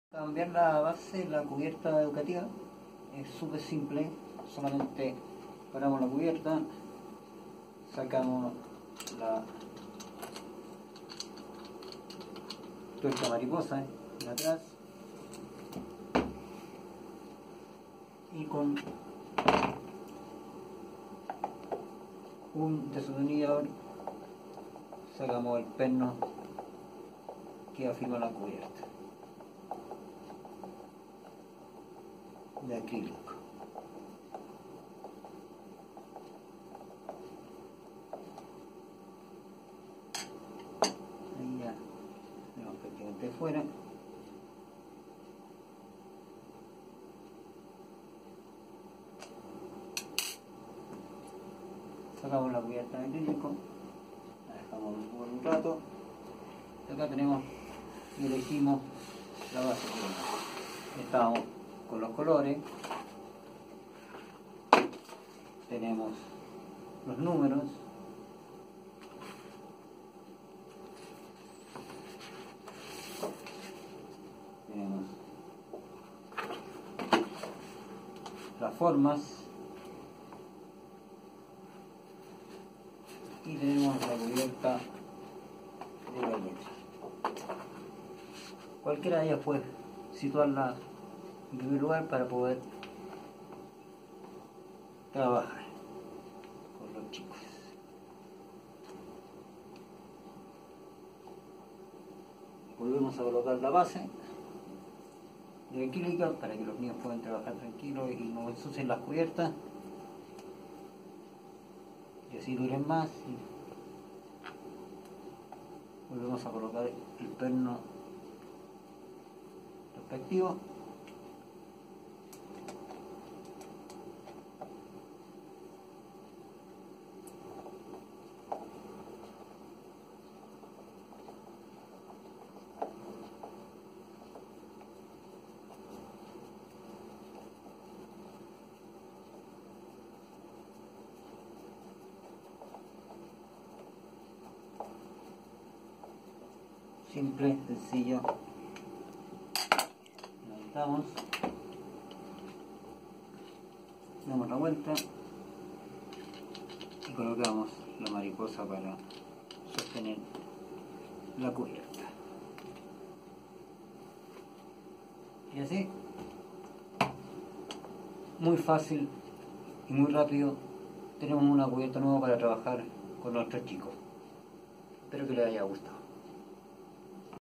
Cubierta educativa
Gracias por interesarte en nuestro complemento educativo. Nosotros estamos ubicados en Santiago de Chile. La base con las cuatro laminas educativas tiene un valor de $ 49000 pesos chilenos. La base más las cuatro laminas y estructura metálica tipo mesa $ 79000 y la base con las cuatro laminas y la estructura metálica tipo mesa más los asientos incorporados $ 119000.

La cubierta educativa es un complemento didáctico para trabajar con niños en las primeras etapas del aprendizaje.

Pueden aprender, trabajar y entretenerse cuatro niños simultáneamente. La base educativa mide 59 centímetros de diámetro. Cuenta con una cubierta de acrílico transparente de 3 mm. y una base en melamina blanca de 15 mm, con un sistema de enganche con perno y tuerca mariposa. Las cuatro laminas que se incorporan a la estructura están confeccionadas en papel fotográfico de 220 gramos impresos en alta resolución.

Cada lamina ha sido creada con el propósito de apoyar el trabajo docente y estimular a los chicos en su aprendizaje, no solo muestra información sino que también desafíos de trabajo.

Nuestros productos no incluyen los costos de despacho.

Saludos cordiales,

Gabriel Berenguela Baeza.